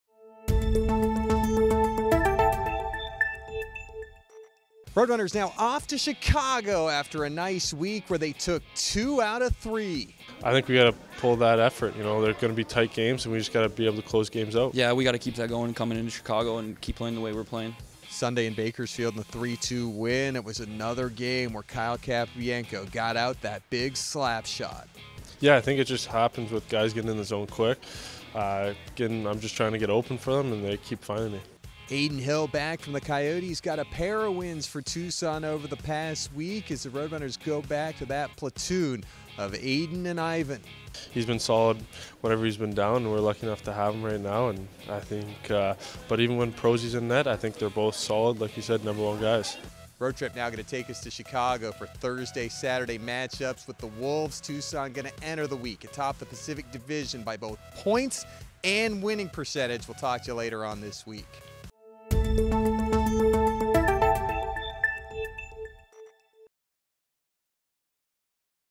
RRTV Weekly: Next Stop Chicago
The Roadrunners head to Chicago after taking two of three this past week with wins against San Diego and Bakersfield as "Voice of the Roadrunners" Adrian Denny catches up with Kyle Capobianco and Kelly Klima who each scored big goals in Sunday's win.